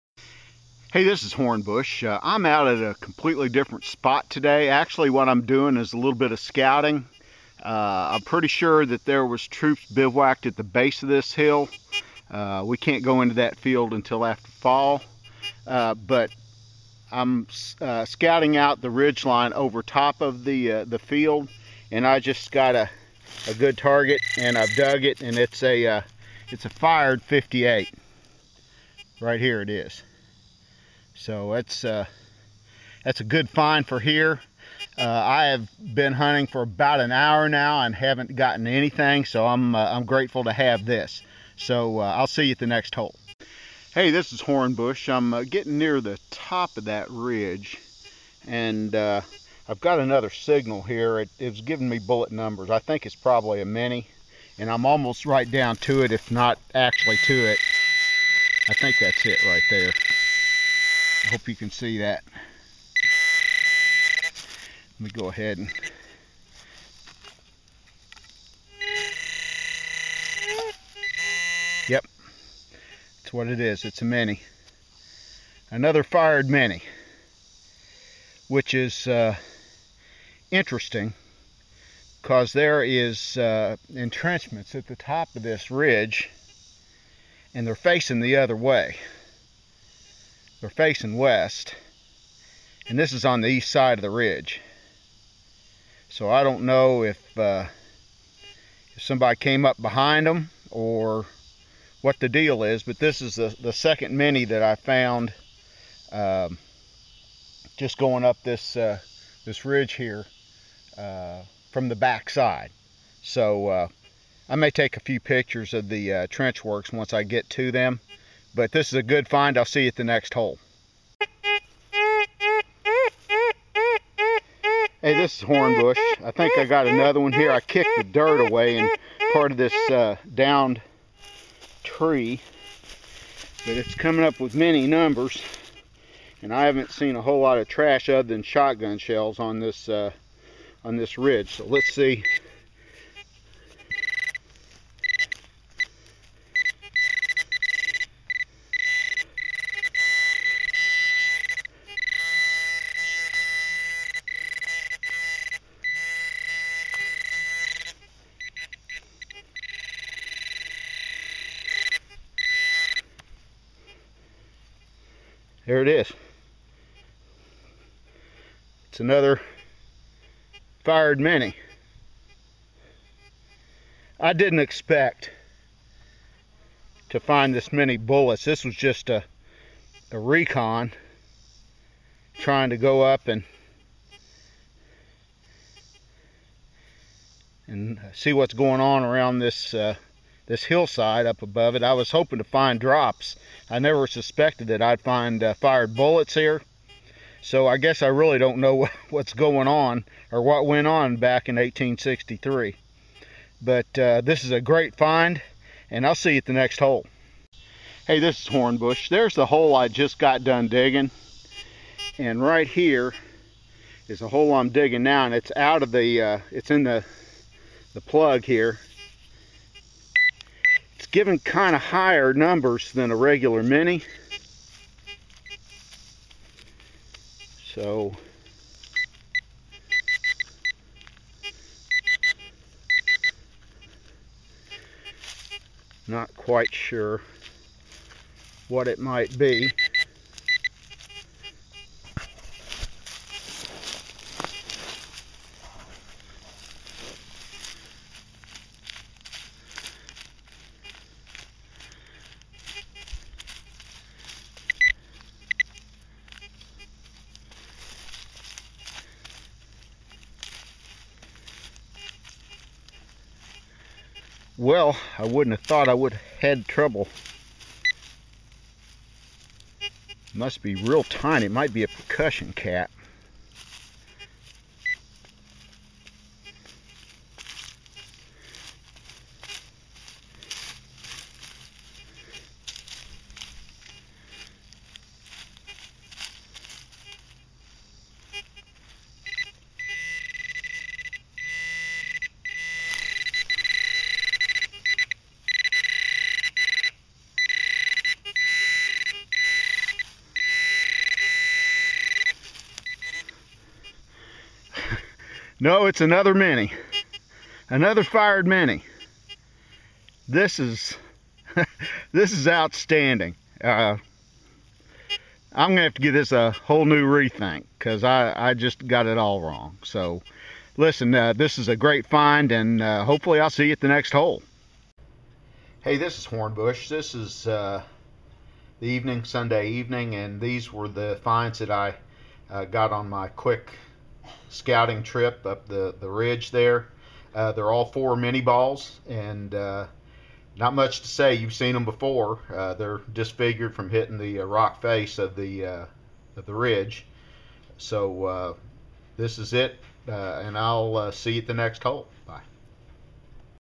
Short Scouting Hunt HB #25
Bob does a 2.5 hour recon of an area he has never hunted before.